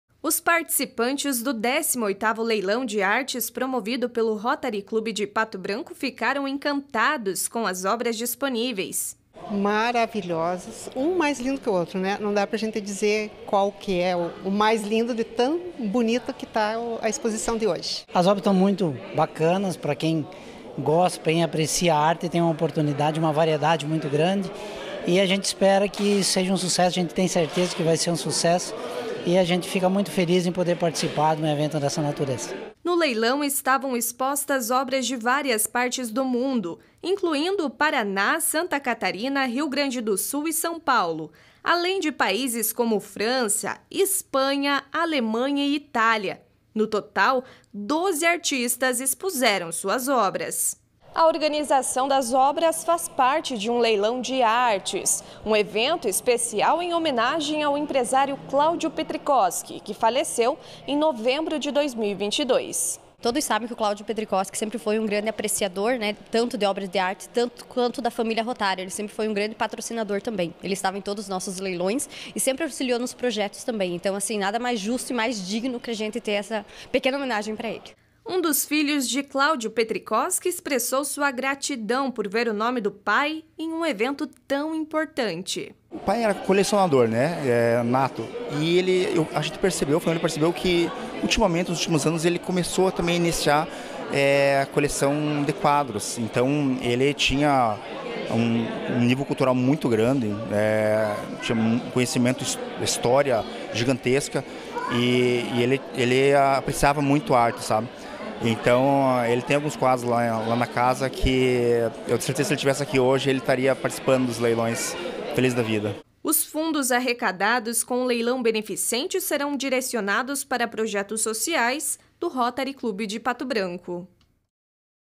Rotary Club de Pato Branco promove XVIII Leilão de Artes e Jantar Beneficente
Eventos: Na última terça-feira (20), o Rotary Club de Pato Branco realizou o XVIII Leilão de Artes e Jantar Beneficente em homenagem ao empresário Cláudio Petrycoski. O evento aconteceu no município de Pato Branco e contou com a presença de diversos participantes.